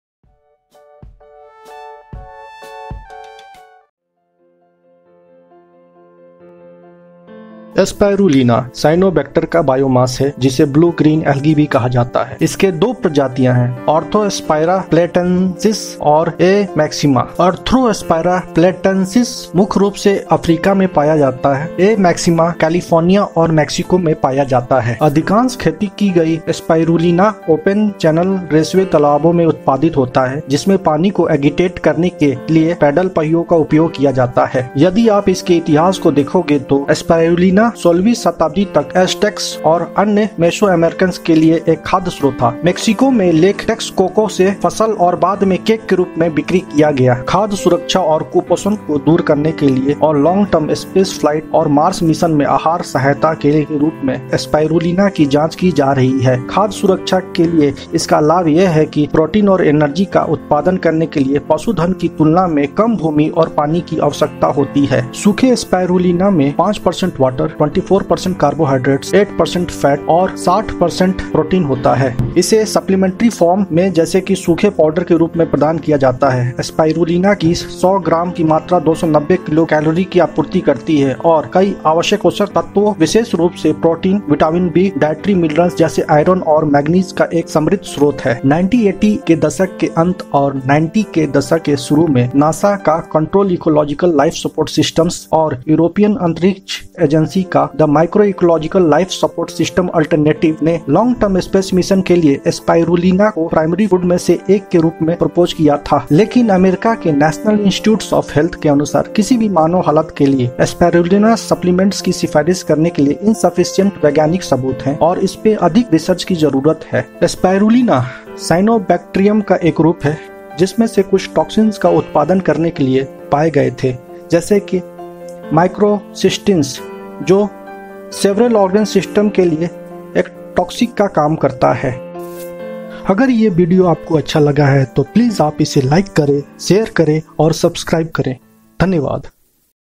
SPIRULINA II What is spirulina ? HINDI . स्पाइरुलिना क्या है?
Spirulina  साइनोबैक्टीरिया का बायोमास है  जिसे  blue-green algae भी कहा जाता है I
आर्थ्रोस्पिरा प्लेटलेटसिस मुख्य रूप से अफ्रीका में पाया जाता है । ए मैक्सिमा कैलिफोर्निया और मेक्सिको में पाया जाता हैI